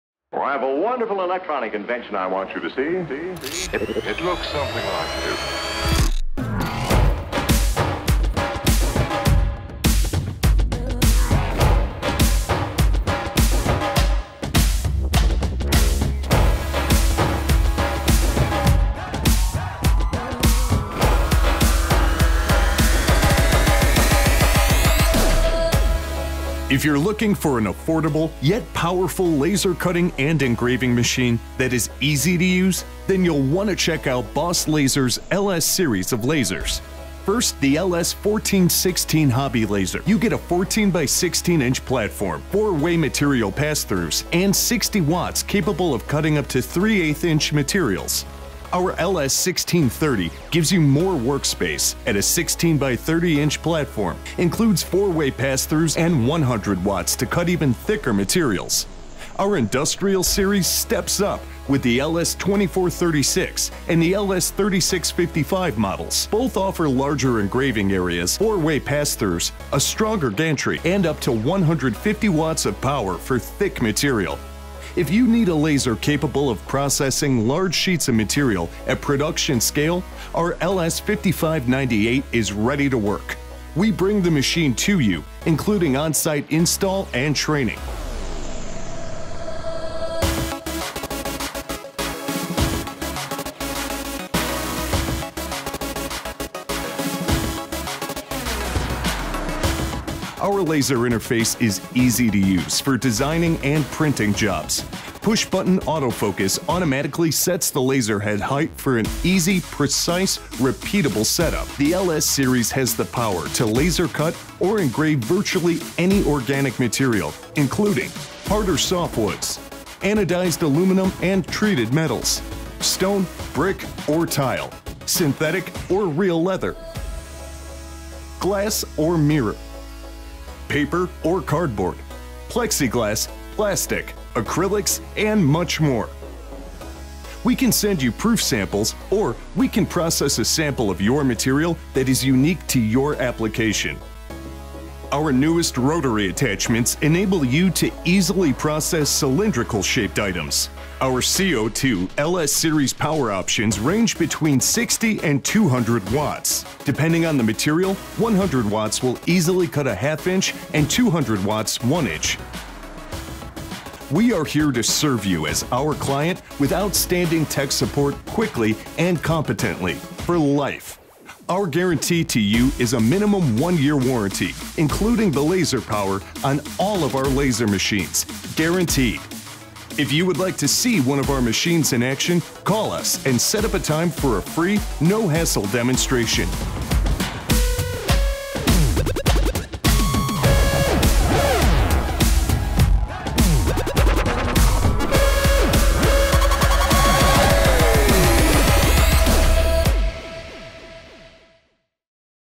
Affordable CO2 Laser Cutter & Engraver Machines - Boss Laser
If you're looking for an industrial grade co2 laser cutter / engraver at an affordable price then the Boss LS SERIES may represent the best value on the market.   Power levels range from 50 to 150 watts capable of cutting up to 1" materials.  Need engraving space the LS offers engraving areas between 14x16" to 55x98" with 4-way material pass thru points.

WHAT CAN OUR LS SERIES LASER CUT or ENGRAVE?

Laser cut, mark or engrave wood, treated metals, acrylics, plastics, leather, fabric, MDF, paper, cardboard, stone, Corian, granite, marble, foam, fiberglass, rubber, anodized aluminum - virtually any organic materials.

Financing options available for any applicants including good, new, and credit challenged applicants.